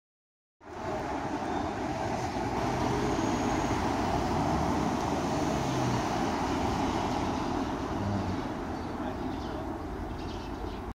Tesla Model 3 Decel
Black Tesla Model 3 w contrasting white multi-spoke alloys, heading to I76.